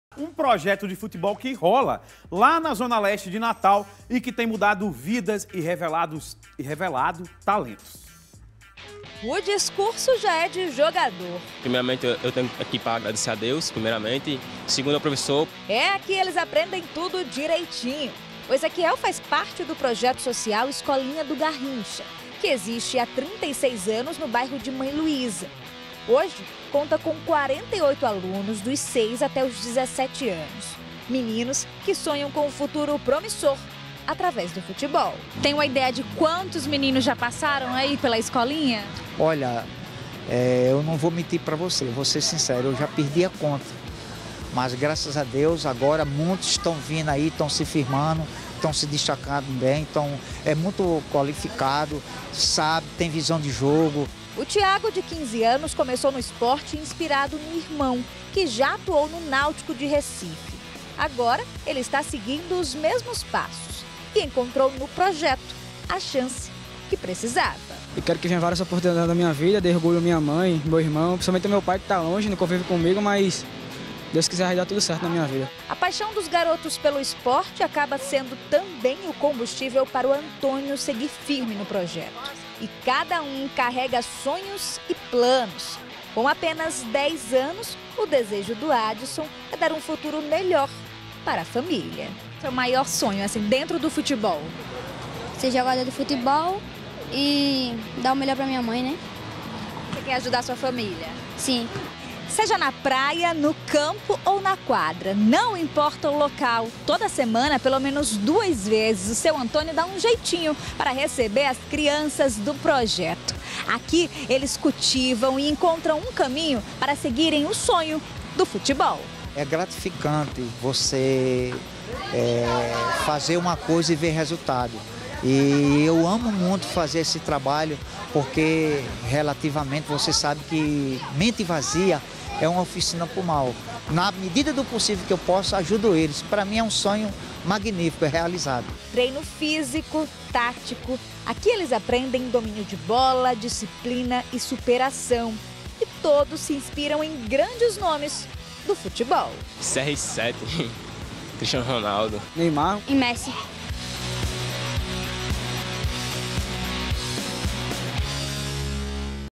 Escolinha de futebol revela craques do futebol em Natal
Escolinha de futebol revela craques do futebol em Natal. Reportagem Thalles Íkaro e Vanessa Florencio. Arena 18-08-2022.